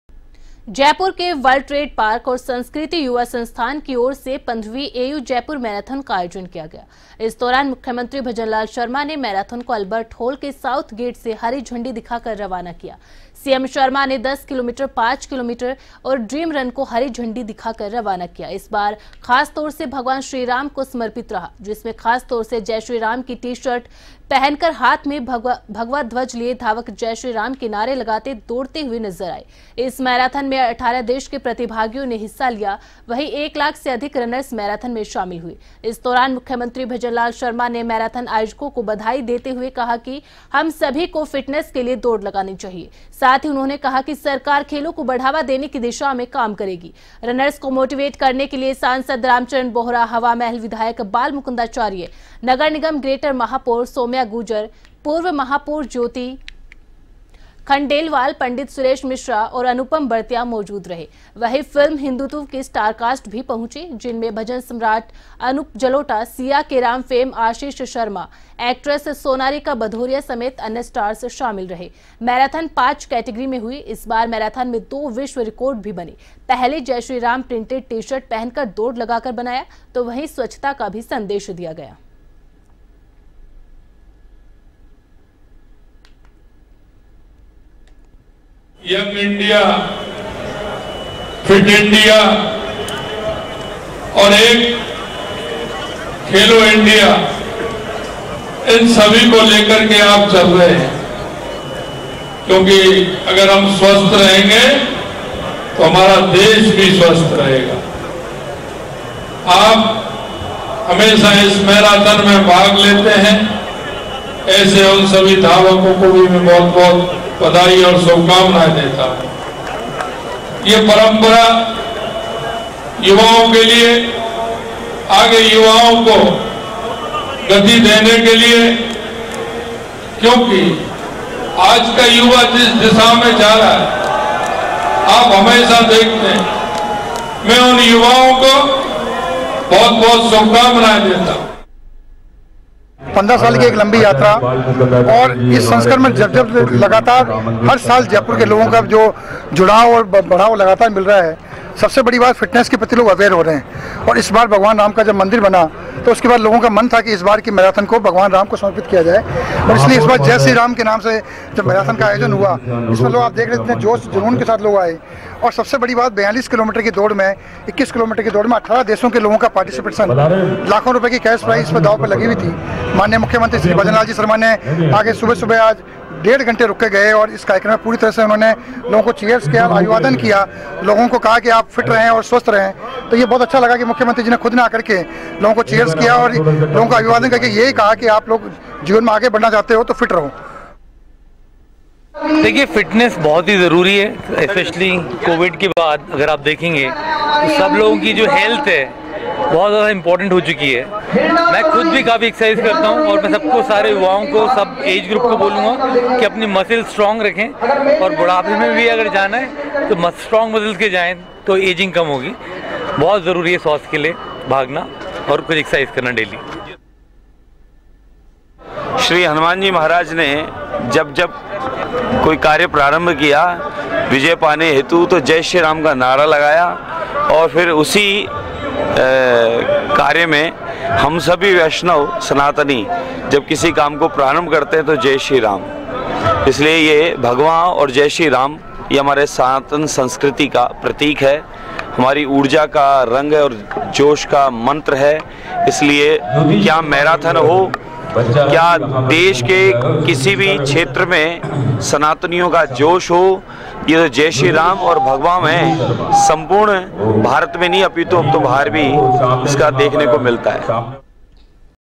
15वीं एयू जयपुर मैराथन का आयोजन, मुख्यमंत्री भजनलाल शर्मा ने मैराथन को दिखाई हरी झंडी
15वीं एयू जयपुर मैराथन का आयोजन, मुख्यमंत्री भजनलाल शर्मा ने मैराथन को दिखाई हरी झंडी

A1TV News Channel :

रविवार सुबह 8:30 बजे और शाम 4:30